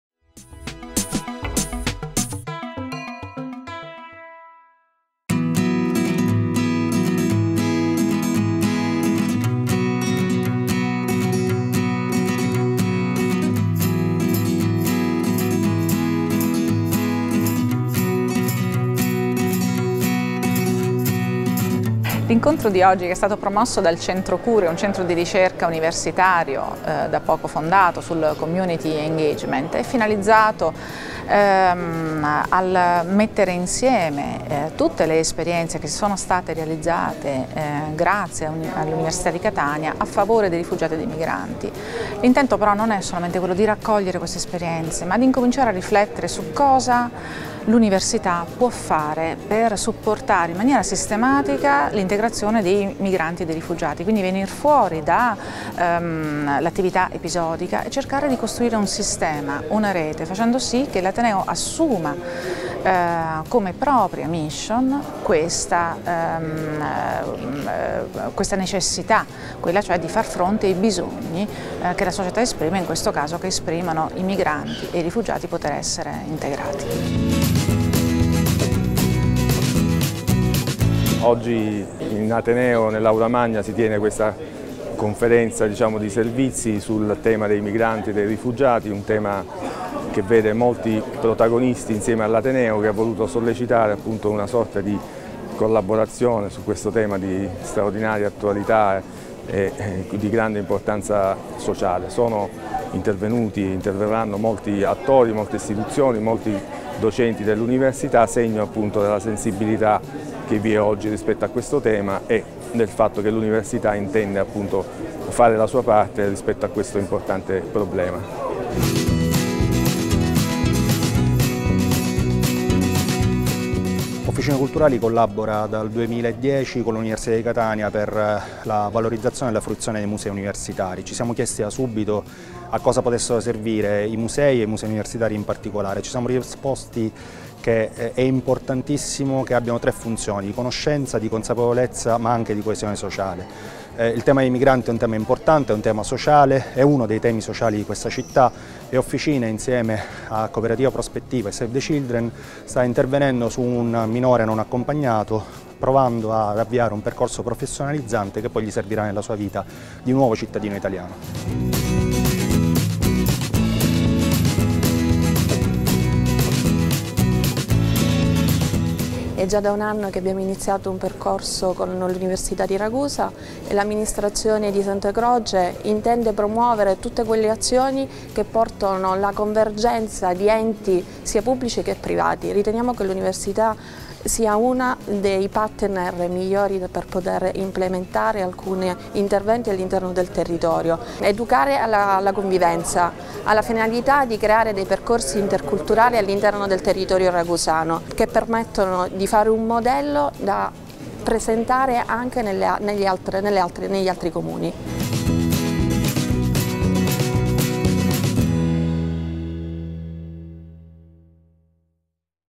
La responsabilità sociale dell’università. L’ateneo catanese per i rifugiati e i migranti
Catania, 4 luglio 2018 - Palazzo centrale, Aula Magna
In video il servizio realizzato da Zammù Tv in occasione del convegno dal titolo "La responsabilità sociale dell’università. L’Ateneo catanese per i rifugiati e i migranti", che si è svolto lo scorso 4 luglio nell'aula magna del Palazzo centrale.
La web tv dell'Università di Catania ha intervistato la prof.ssa Roberta Piazza, presidentessa del centro di ricerca di Ateneo sul Community Engagement (CUrE), che ha promosso l'iniziativa, il prorettore Giancarlo Magnano San Lio, il presidente dell'associazione "Officine culturali" Francesco Mannino e l'assessore ai Rapporti con l'Università del Comune di Santa Croce Camerina Giulia Santodonato.
"L'incontro - ha spiegato in particolare la prof.ssa Piazza - è finalizzato a mettere insieme tutte le esperienze che sono state realizzate grazie all'Università di Catania a favore dei migranti. E' inoltre un'occasione per cominciare a riflettere su cosa l'Ateneo può fare per supportare in maniera sistematica l'integrazione di migranti e di rifugiati". Per la docente è necessario "costruire un sistema e una rete, facendo sì che l'Ateneo assuma come mission quella di aiutare i migranti nel loro processo di integrazione".

Roberta Piazza - Università di Catania
Giancarlo Magnano San Lio - Prorettore Unict
Francesco Mannino - Officine culturali
Giulia Sandonato - Assessore Comune Santa Croce Camerina